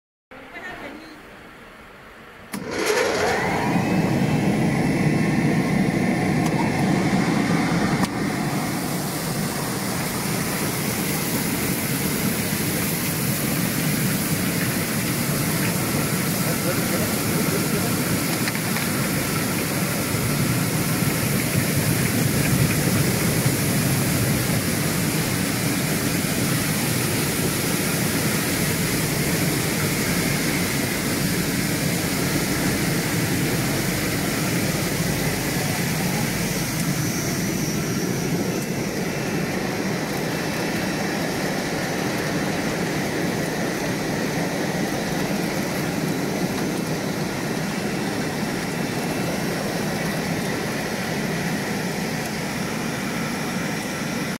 Bubble Washing Machine / Vegetable Washing Machine / Fruit Washer / Bubble Washer
Our machines are made of SUS 304 stainless steel which is safe and durable, and can be used for long time. It is suitable for processing various kinds of food items.